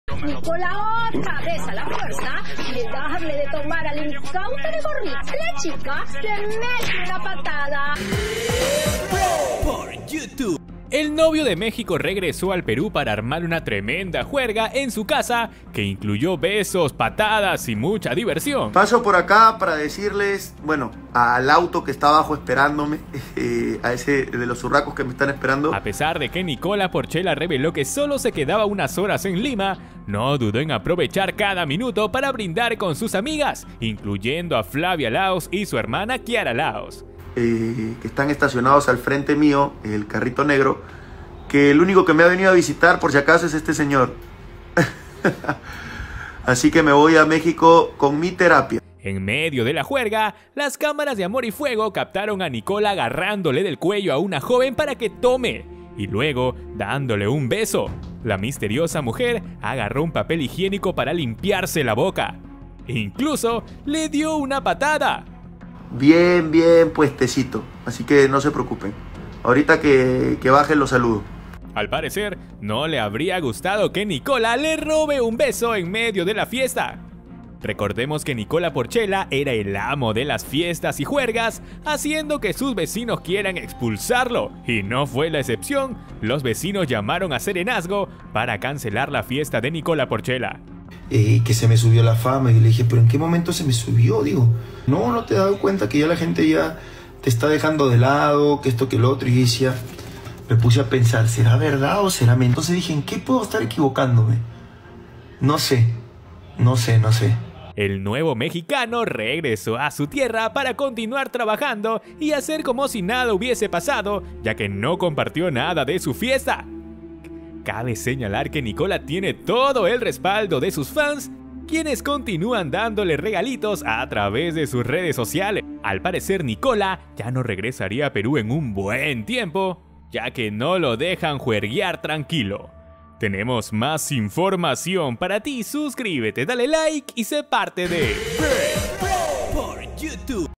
NICOLA PORCELLA BESA A MUJER Y LO RECHAZAN ¡LO EMPUJAN POR CONFIANZUDO!
Nicola Porcella organiza fiesta en su casa y su conquista no salió como esperaba.